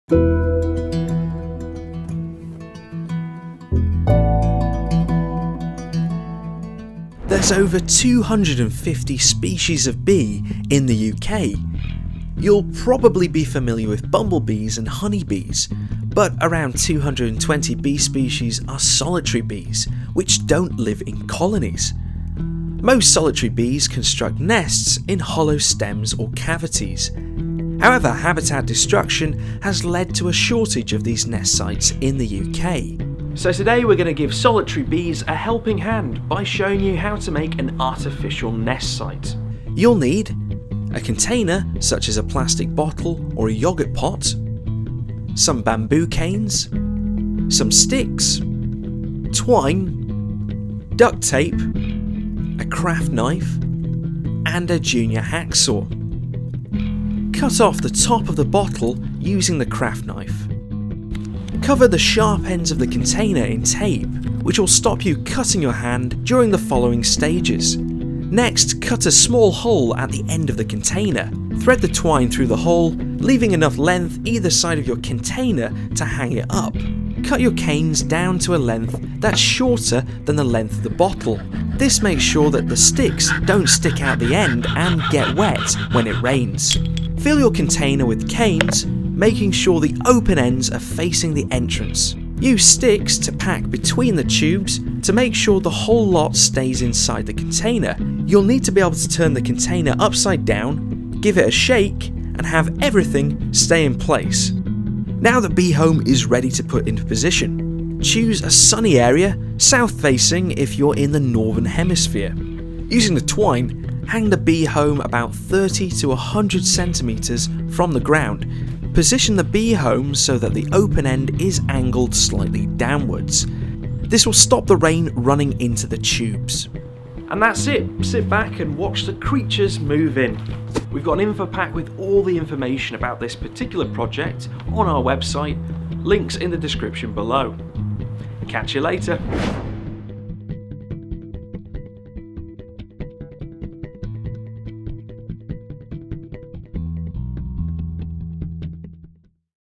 How to make a solitary bee home
Did you know there are over 200 solitary bee species in the UK. Unfortunately over the past 40 years, habitat destruction has mean't their homes have vanished. So why not give them a helping hand by building an artificial bee home?